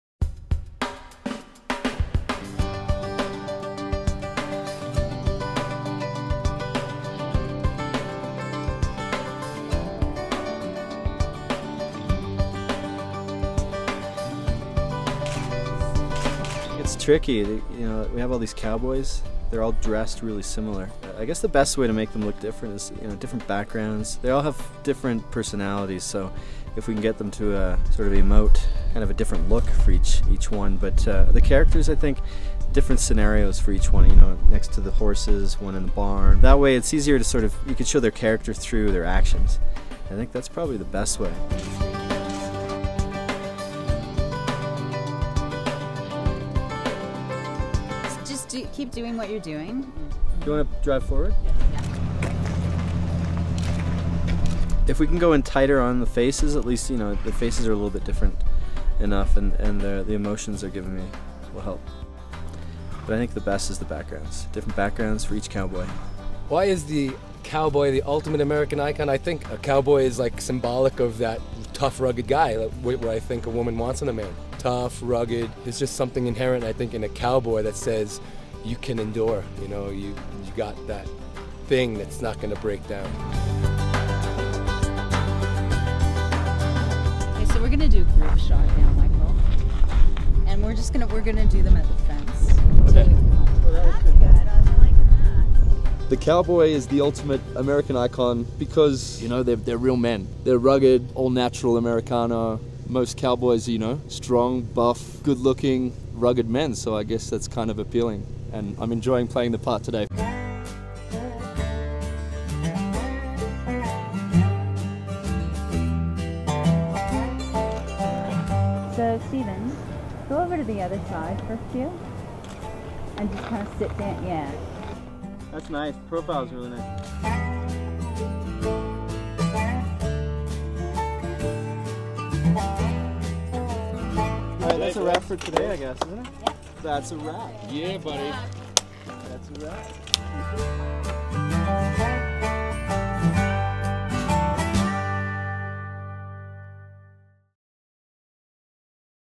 The Creeds Webisode THREE: Behind the Scenes at a Harlequin Cover Shoot
Experience the thrill of the Creed Family and Stone Creek series in an incredible new trilogy from New York Times and USA TODAY bestselling author Linda Lael Miller.  Watch our exclusive webisode on Behind the Scenes at a Harlequin Cover Shoot

Webisode 3:
Watch as photographer Michael Alberstat selects appropriate locations and poses for all three cowboys to give each book its own unique flavor.